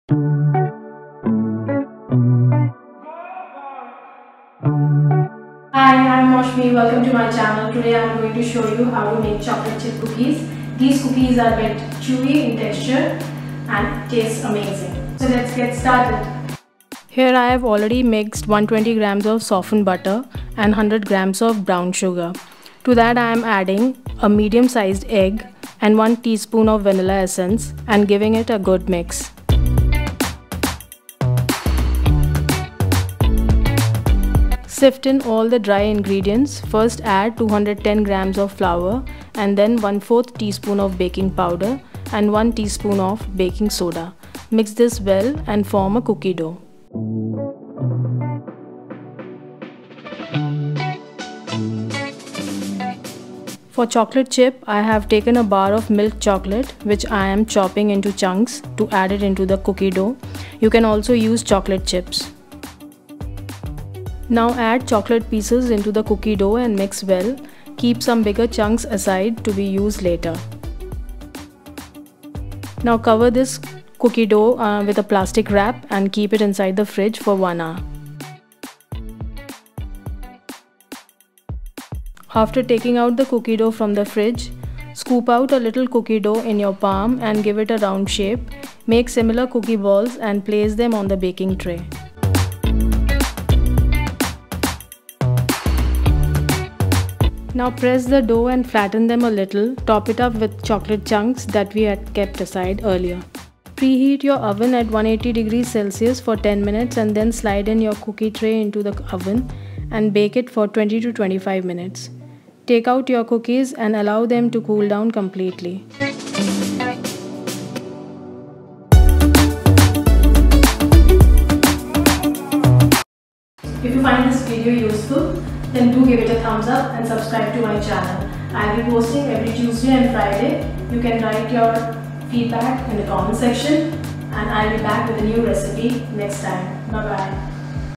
Chocolate Chip Cookies | The Best Chocolate Chip Cookies Recipe 🍪
Hey everyone,

This chocolate chip cookie recipe is easy to make and tastes amazing, especially, the big chocolate chunks you get in every single bite is hard to resist.

Ingredients Used-

Unsalted Butter- 120gms
Brown Sugar- 100gms
Medium Sized Egg- 1
Vanilla Essence-1tsp
All Purpose Flour- 210gms
Baking Powder-1+1/4tsp
Baking Soda- 1tsp
Chocolate Chips or Chunks- 150gms
Salt(optional)- 3gms